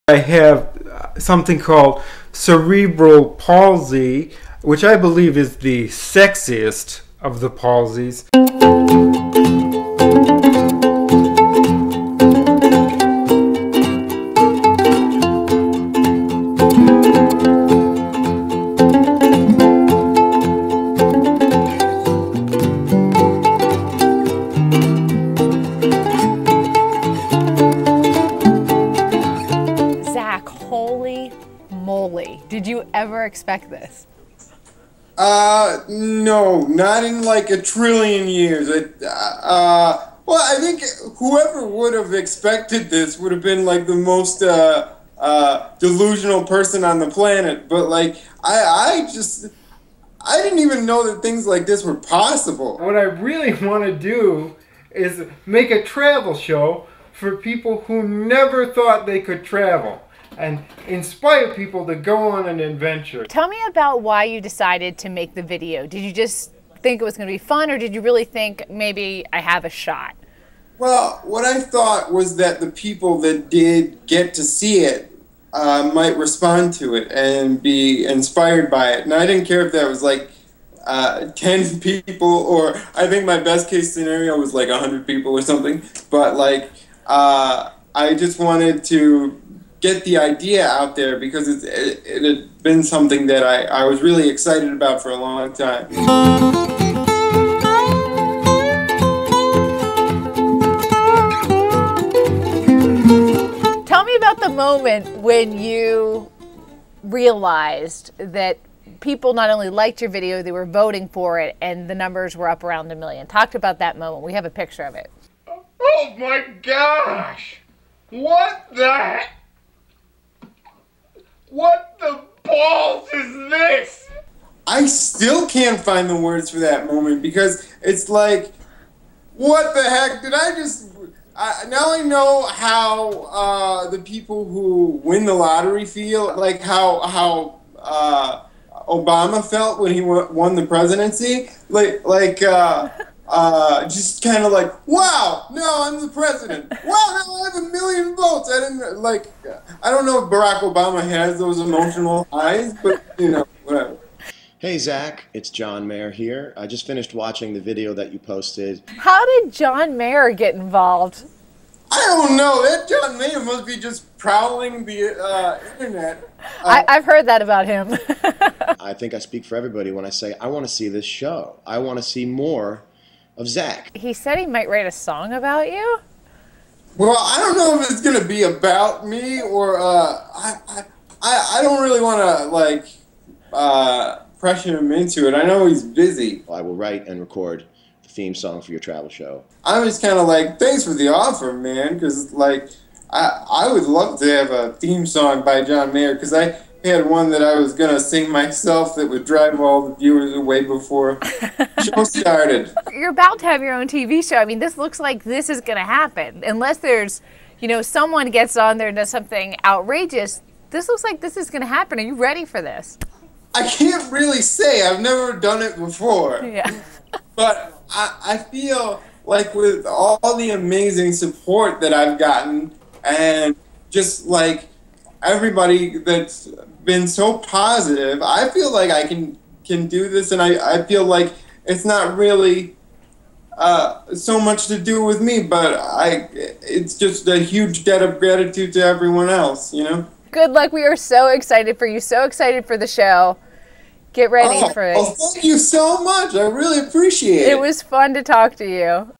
The Conversation: Zach Anner Oprah's Unlikely Star
ABC's Sharyn Alfonsi talks with the Web star who could win his own show.